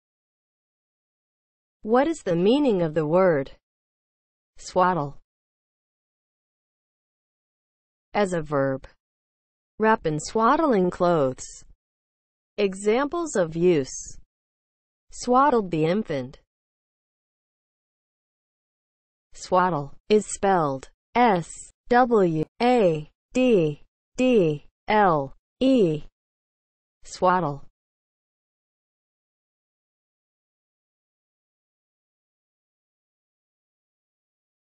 What is the meaning of the word SWADDLE?
Definition, examples of use and spelling of the word SWADDLE.

(00:00) Intro
(00:07) As a verb
(00:18) Spelling

As a verb (00:07):
• wrap in swaddling clothes

Examples:
• "swaddled the infant"